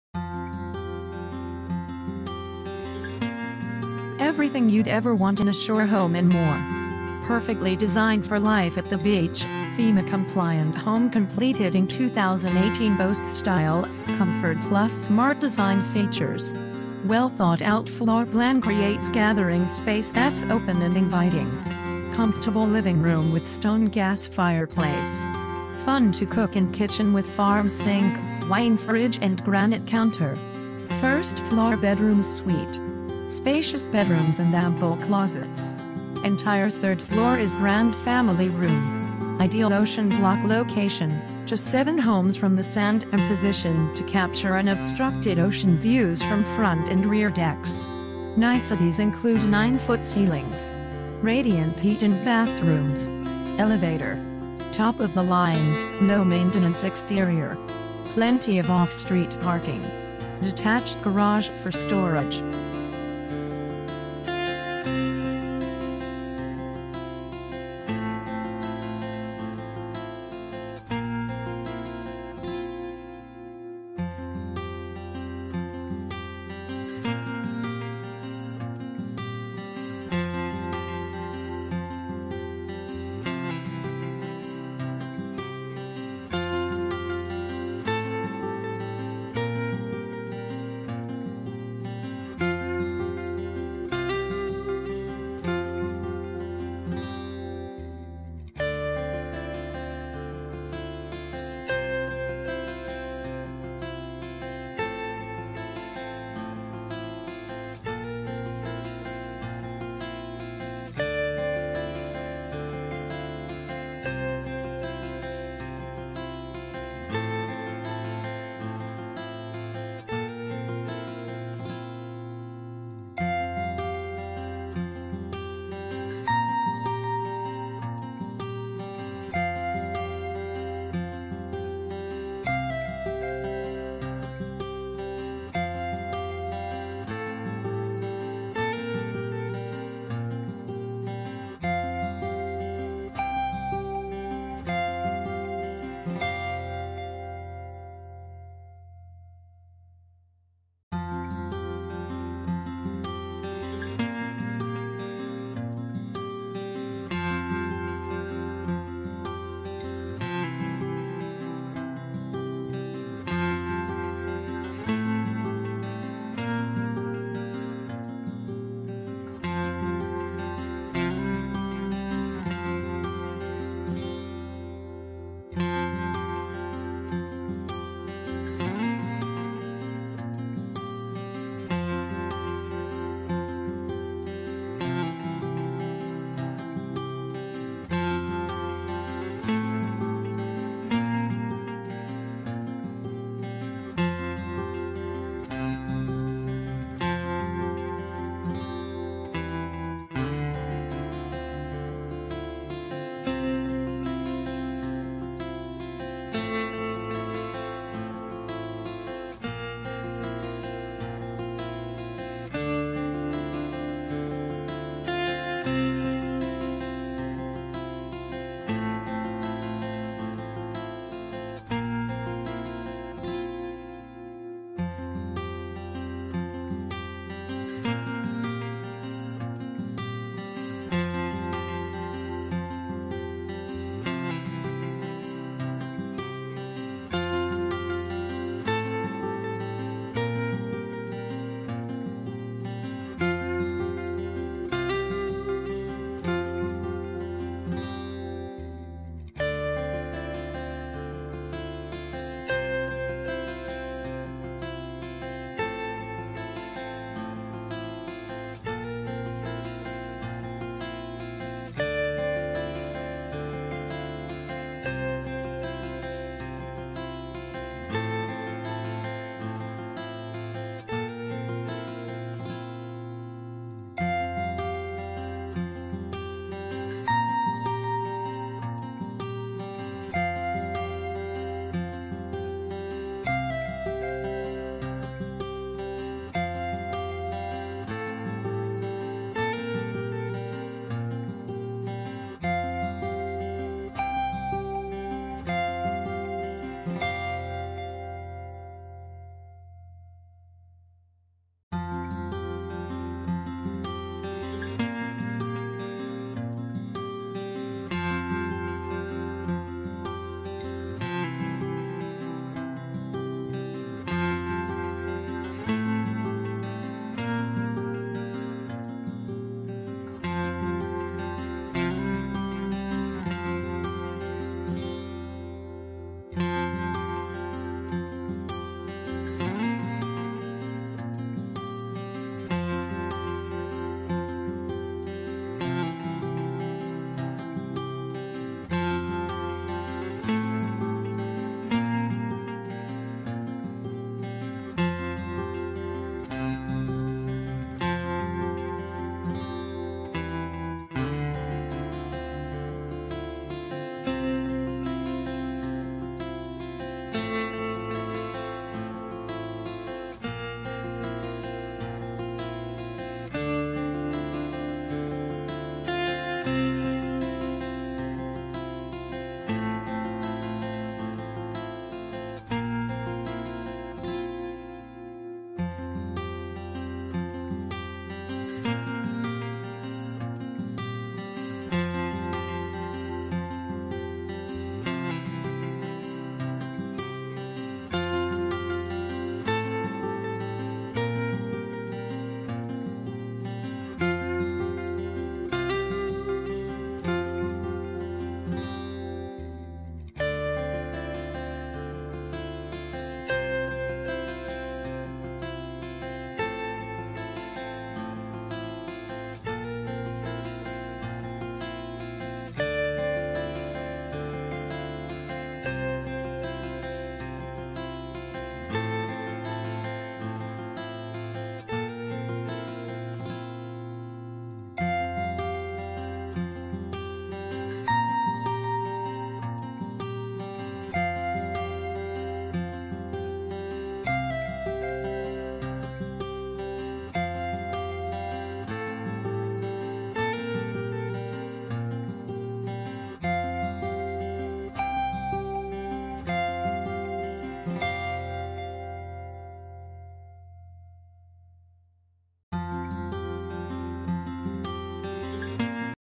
39 5th Ave | Laurie Enoch
Everything you'd ever want in a shore home and more! Perfectly designed for life at the beach, FEMA compliant home completed in 2018 boasts style, comfort + smart design features. Well-thought-out floor plan creates gathering space that's open & inviting. Comfortable living room with stone (gas) fireplace. Fun-to-cook-in kitchen with farm sink, wine fridge & granite counter. 1st fl. bedroom suite. Spacious bedrooms & ample closets. Entire 3rd fl. is grand family room. Ideal OCEAN BLOCK location--just 7 homes from the sand and positioned to capture UNOBSTRUCTED OCEAN VIEWS from front & rear decks. Niceties include 9 ft. ceilings. Radiant heat in bathrooms. Elevator. Top-of-the-line, low-maintenance exterior. Plenty of off-street parking. Detached garage for storage. Flood ins.=$417.